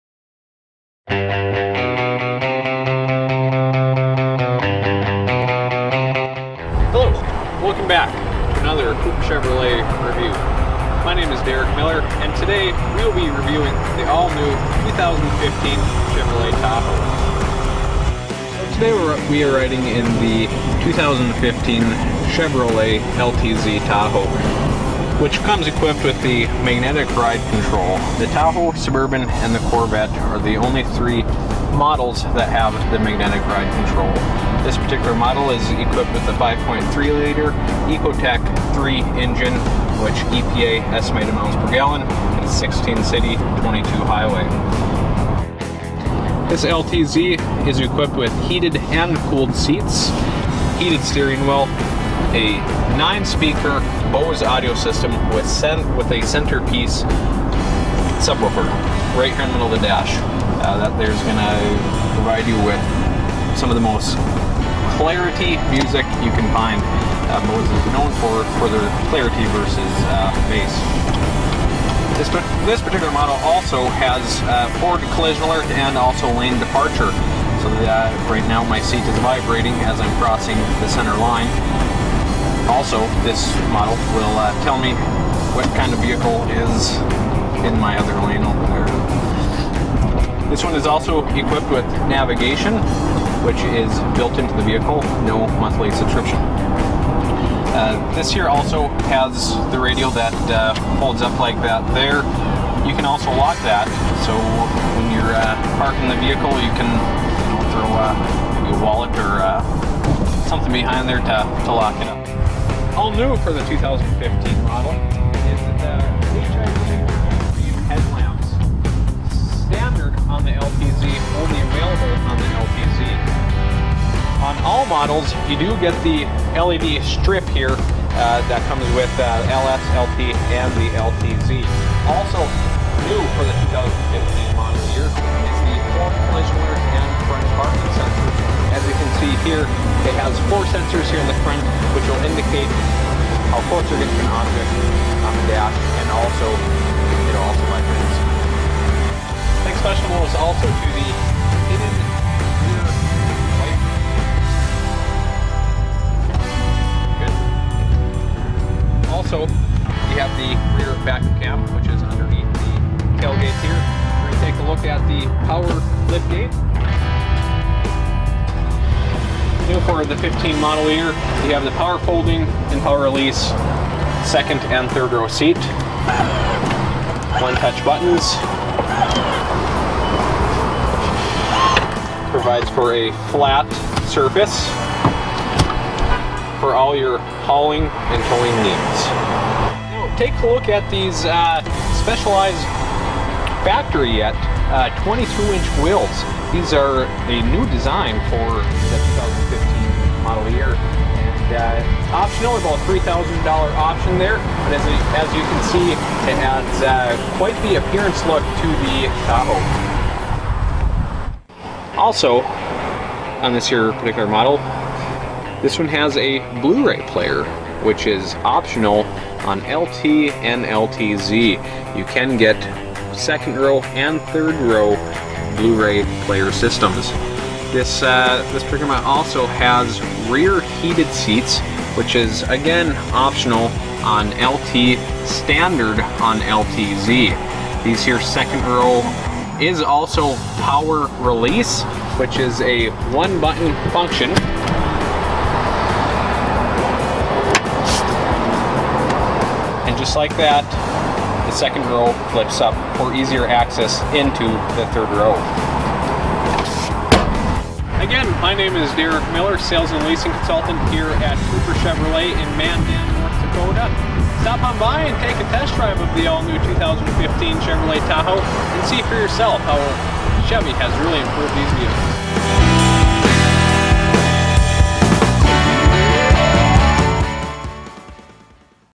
2015 Chevrolet Tahoe | Take the Wheel Tuesday
This week's TaketheWheelTuesday features the all new 2015 Chevrolet Tahoe. Stop in today to test drive this amazing ride yourself.